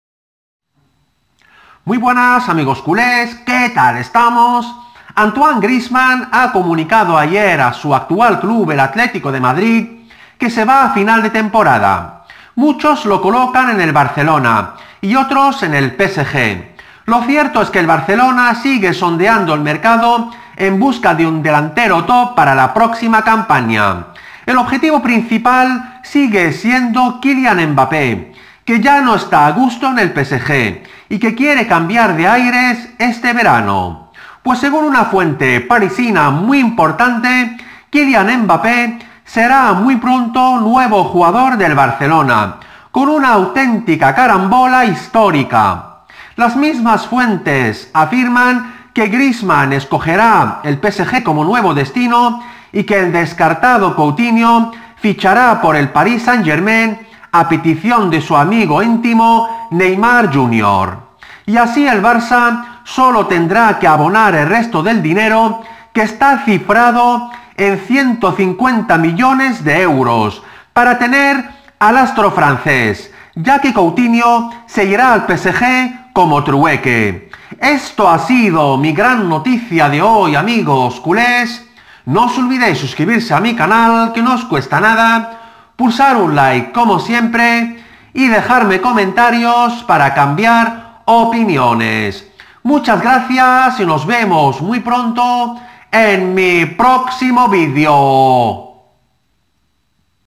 ÚLTIMA HORA: ¡¡EL BARCELONA FICHA A KYLIAN MBAPPÉ CON UNA CARAMBOLA HISTÓRICA!!
Kylian Mbappé será nuevo juagdor del Barcelona con una carambola histórica